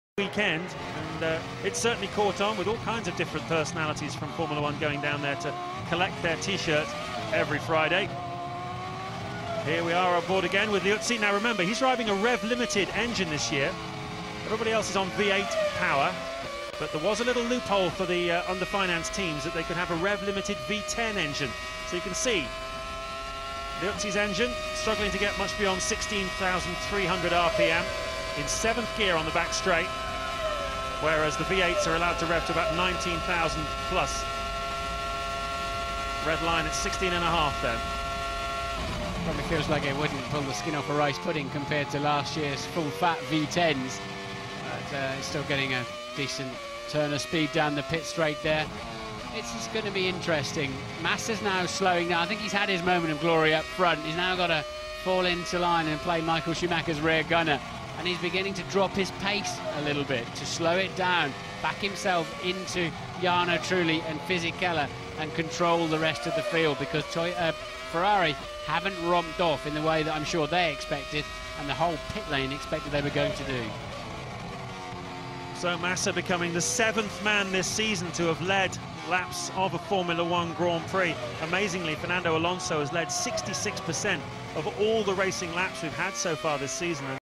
F1 Liuzzi Onboard USA 2006 Scuderia Toro Rosso STR1
Vitantonio Liuzzi 2006 USA onboard camera. V10 engine at low rpm, the sound is awesome. :)